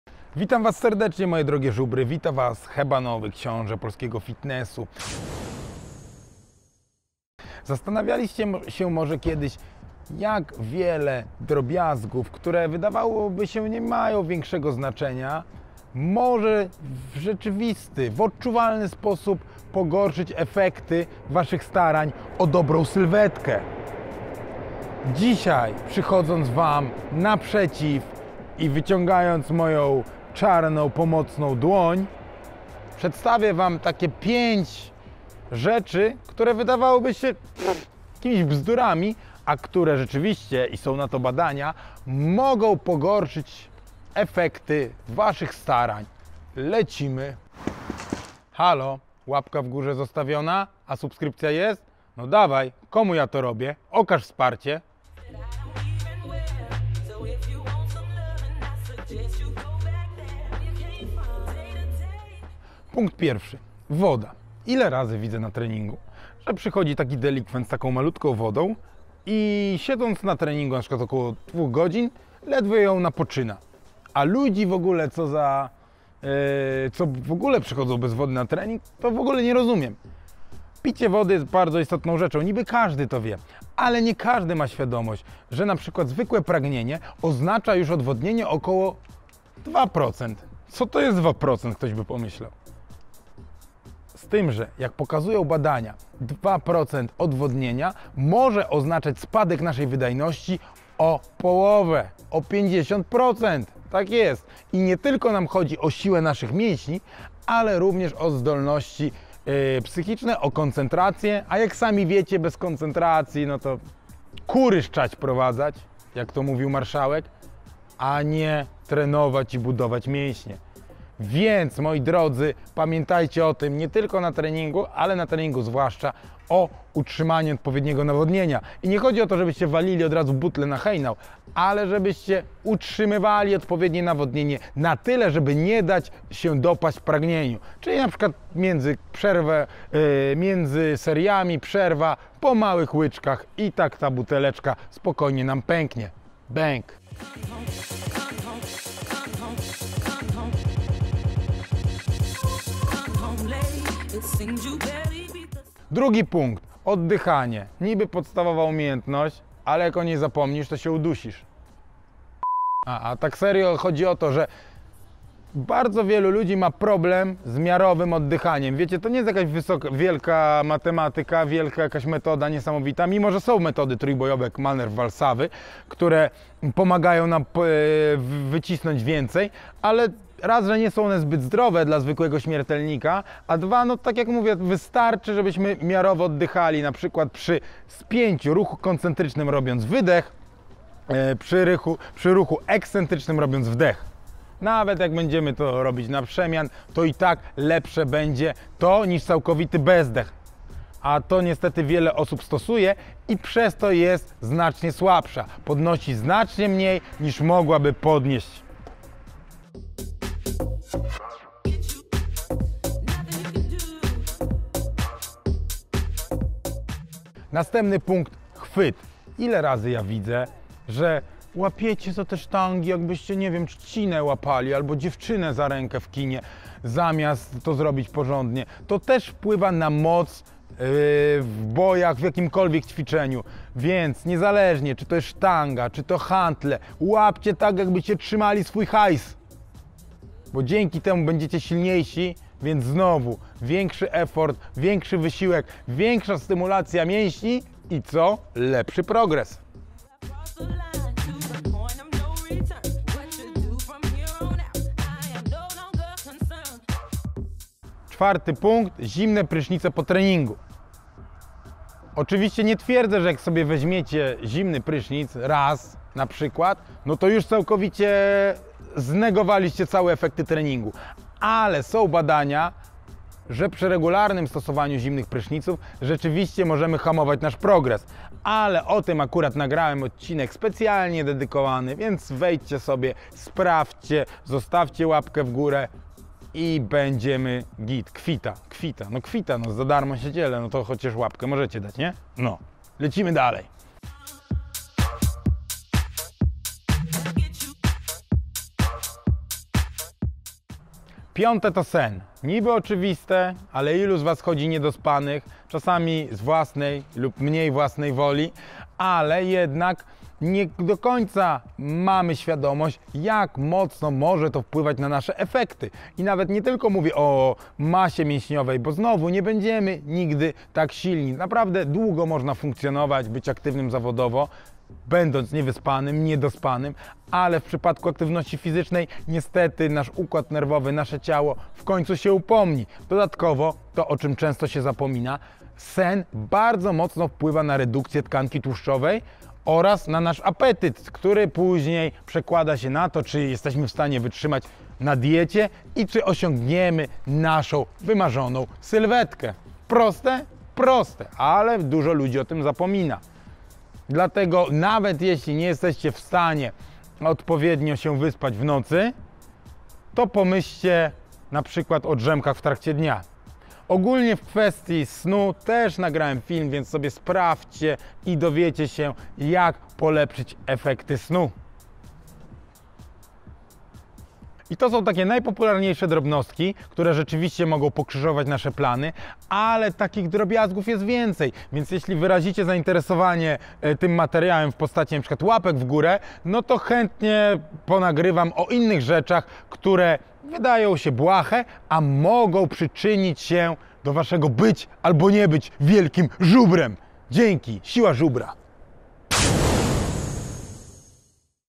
5 "drobiazgów", które hamują Twoje postępy!
woda, chwyt, oddychanie i inne rzeczy które blokuja Twoje postępy